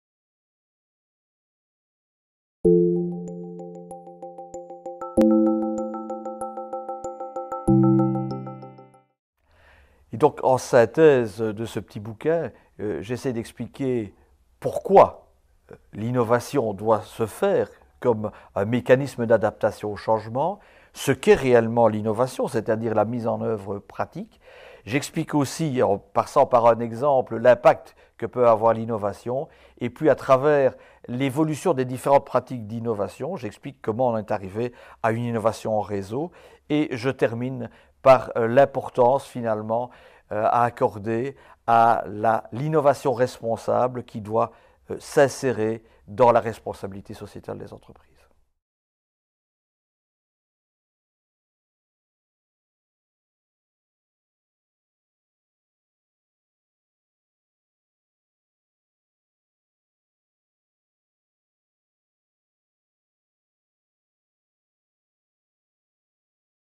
« Pourquoi l’innovation ? Quels défis pour l’Europe ? » (présentation)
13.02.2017
Luc Chefneux et Jacques Olivier